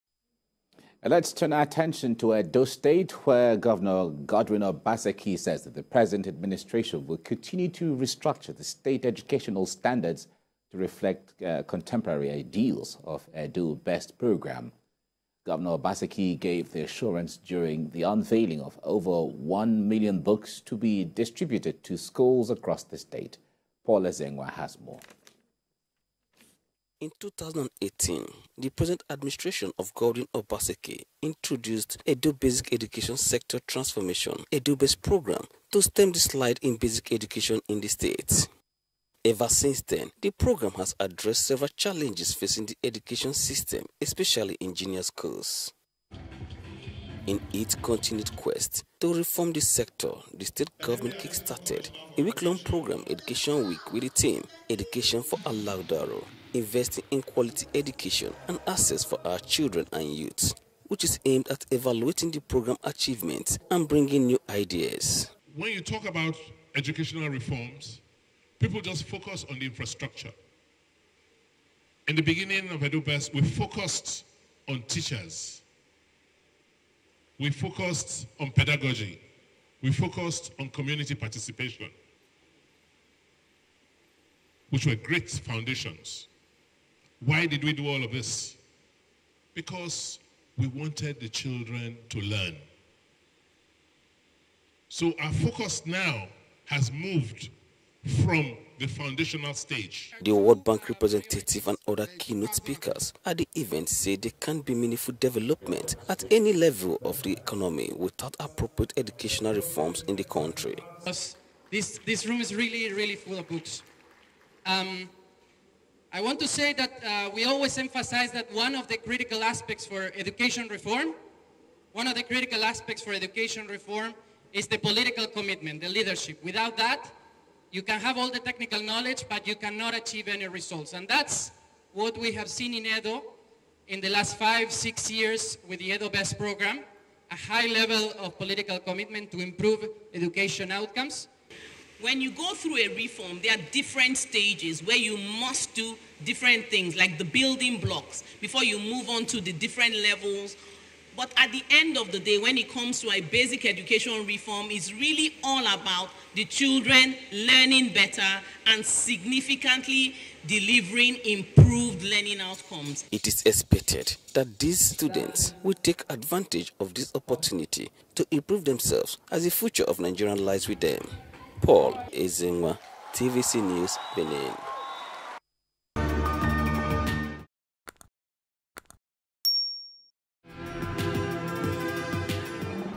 Edo Education Reform: 1.5 Million Books For Distribution To Schools
Godwin Obaseki says the present administration will continue to restructure the state educational standards to reflect the contemporary ideals of the Edo Best programme.